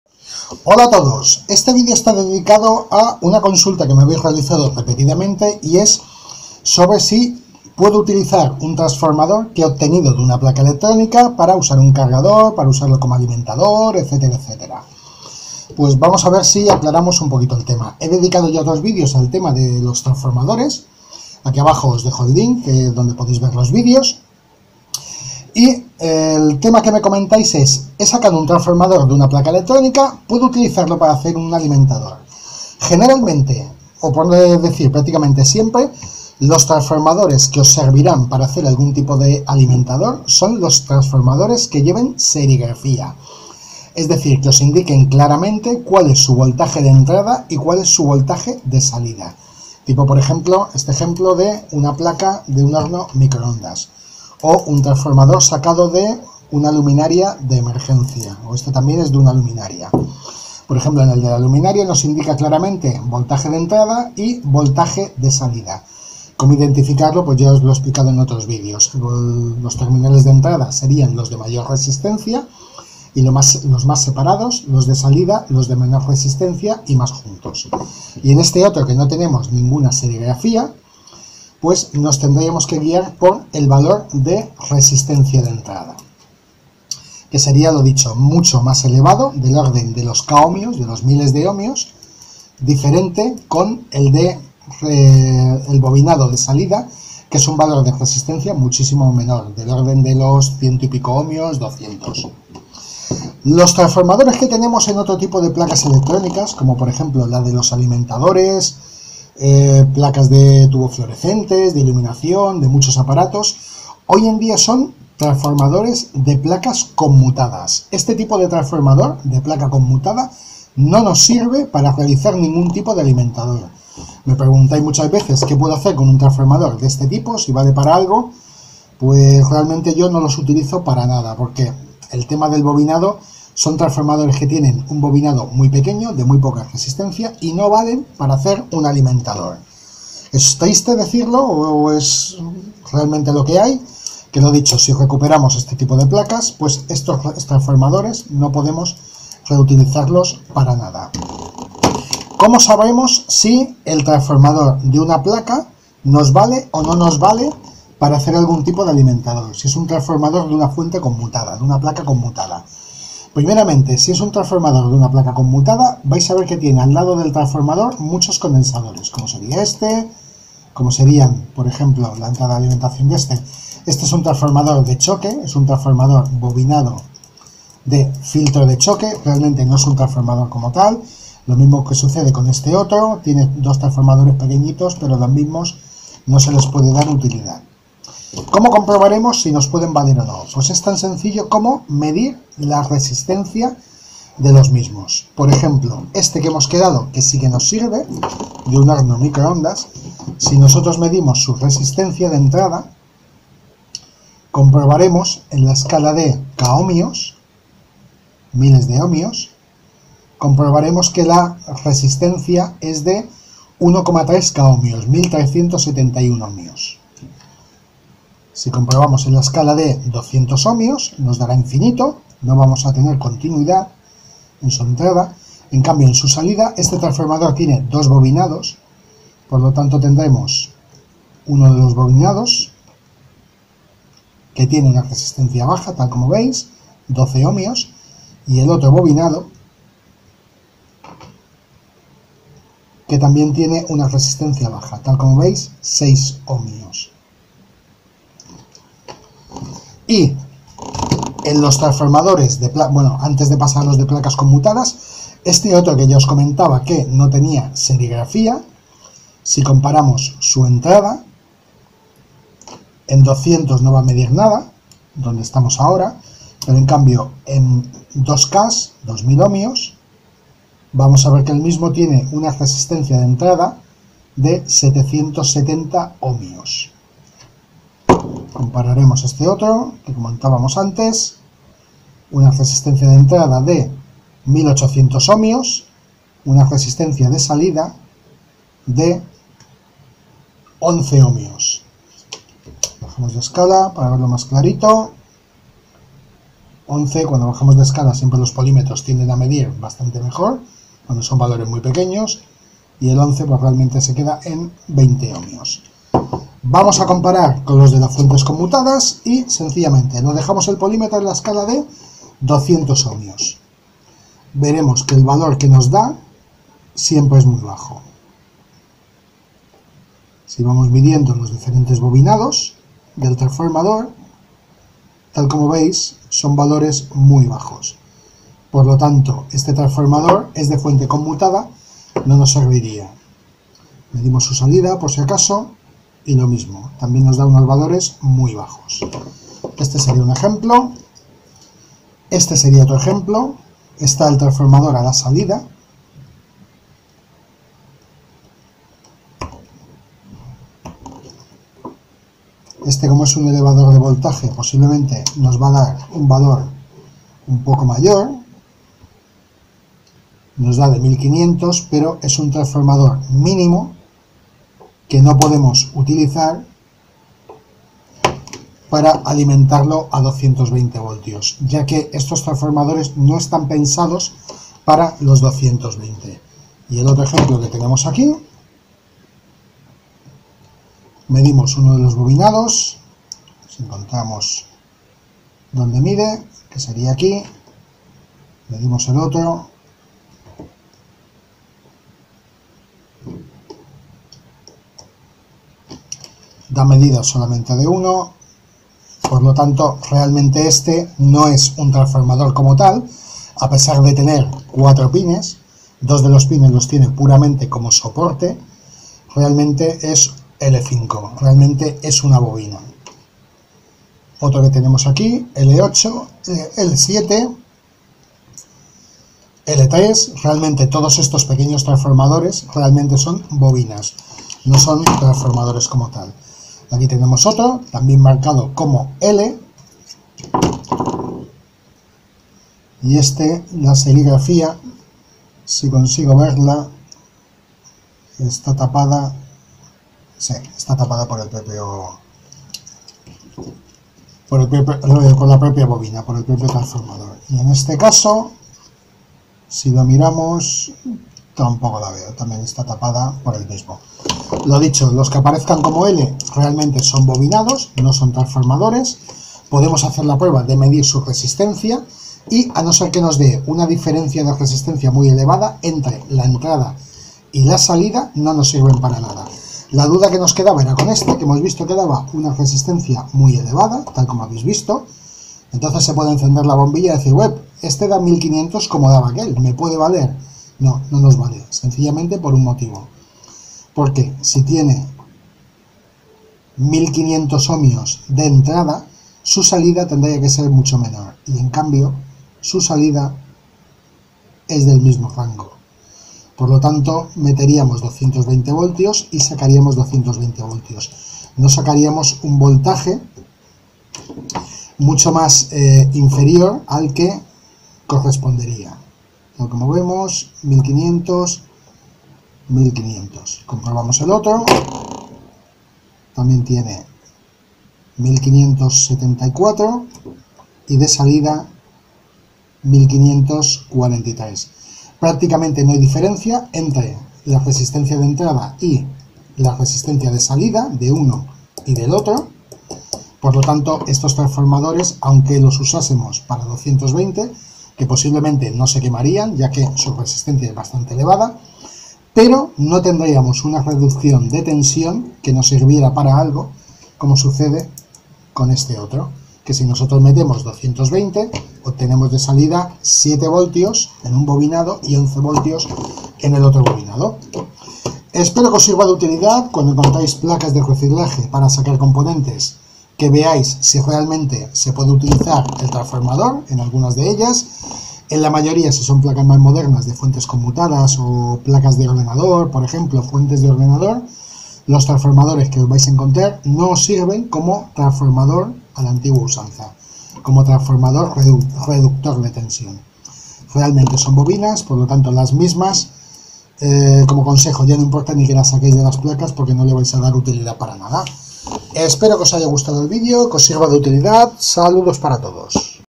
¿QUE TRAFOS PUEDO REUTILIZAR DE PLACAS ELECTRONICAS PARA OTROS USOS?
Una consulta que me habeis realizado en diversas ocasiones ¿puedo reutilizar un trafo de una fuente conmutada o similar?
estos trafos no funcionan a voltaje de red eléctrica directa, ya sea 120 o 230 V según el país.